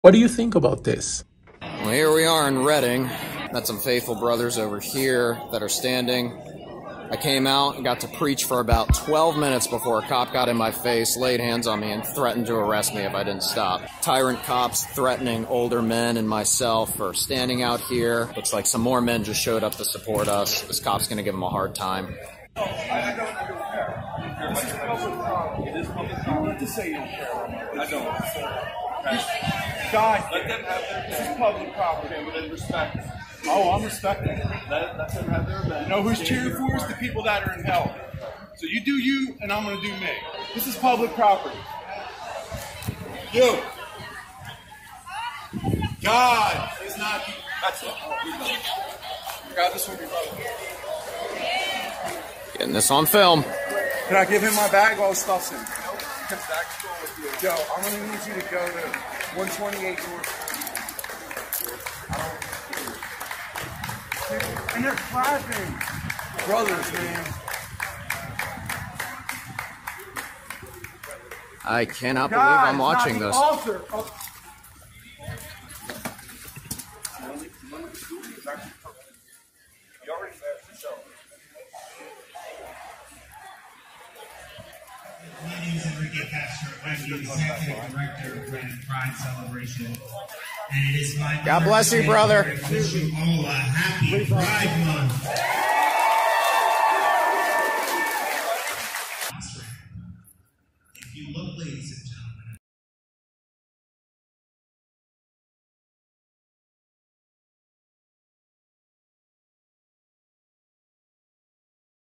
Christian Man Arrested For Quoting The Bible At Pride Event in Pennsylvania
A Christian man in Pennsylvania has been arrested for reading the Bible.